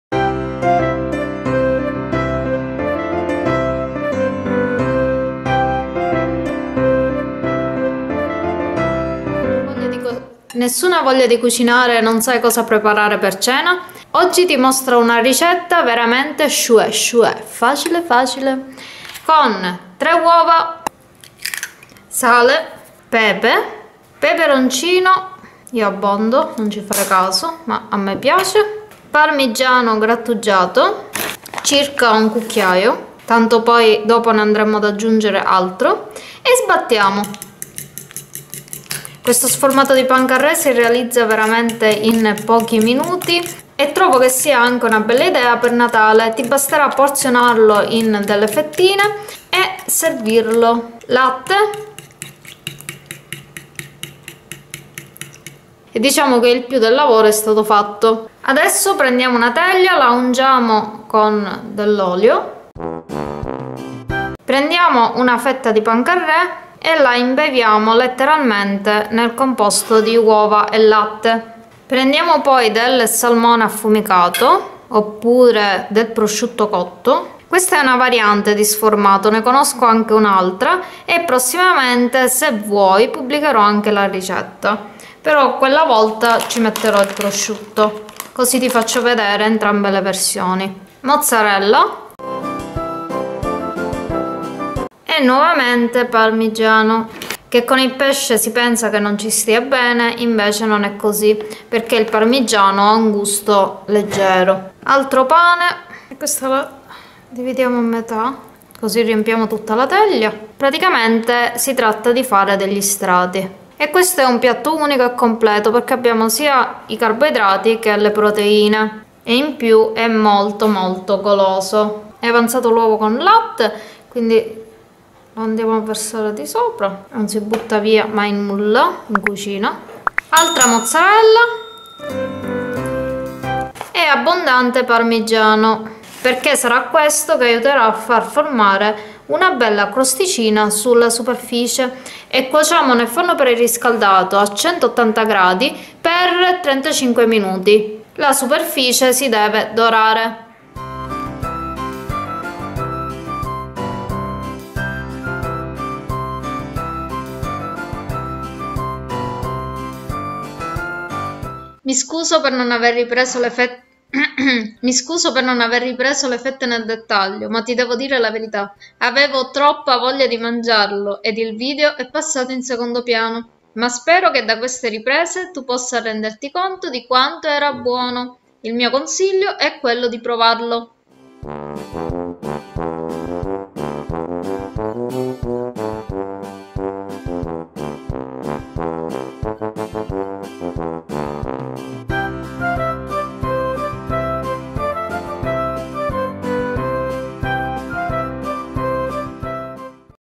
SFORMATO Di Pancarrè Gustoso - Torta Salata Con Salmone RICETTA SPRINT
SFORMATO Di Pancarrè Gustoso - Torta Salata Con Salmone RICETTA SPRINT e facile per una cenetta con i fiocchi infatti se,questa sera non sai cosa portare in tavola e non hai voglia di cucinare,questo sformato salverà la situazione,ti basteranno infatti pochi ingredienti per realizzare un pasto completo e sostanzioso,non ti resta quindi che guardare il video per scoprire la ricetta e provarlo

ti aspettiamo💋. !!!